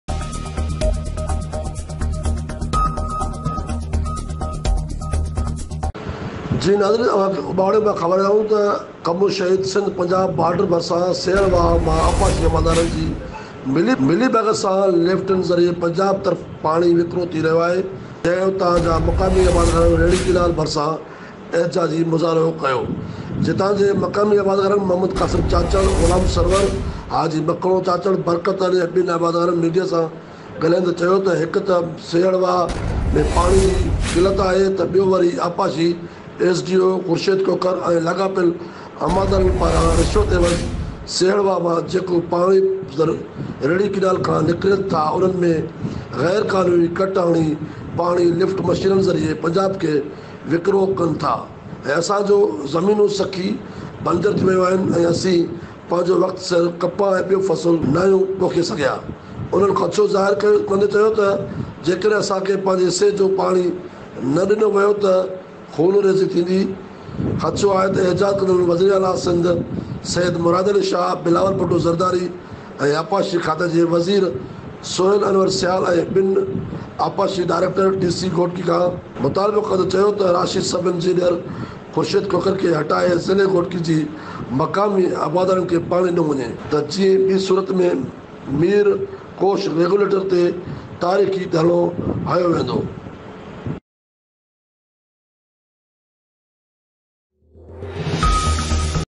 Today|BreakingNews|SDN NEWS
,ڪمون شهيد سنڌ پنجاب بارڊر ڀرسان سيهڙ واه مان آپباشي عملدارن جي ملي ڀڳت سان لفٽن ذريعي پنجاب طرف پاڻي وڪرو ٿي رهيو